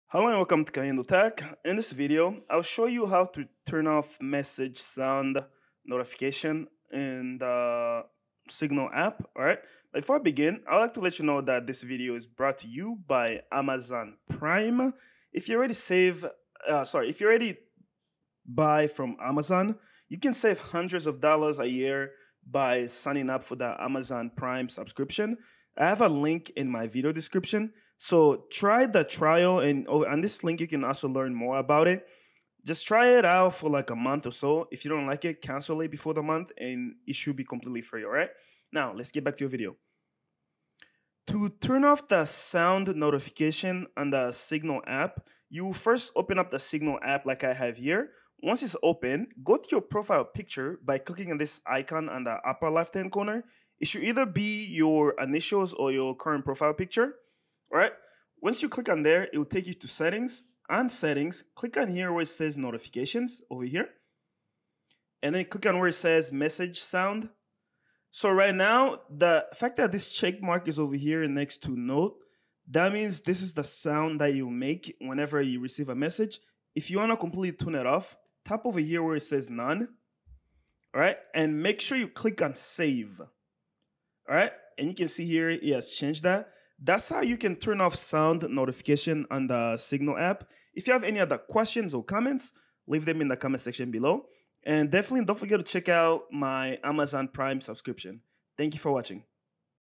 How To Turn Off Message Sound Notification Signal iPhone
This video shows How To Turn Off Message Sound Notification Signal iPhone.

Amazon Deals
- Amazon Prime Trial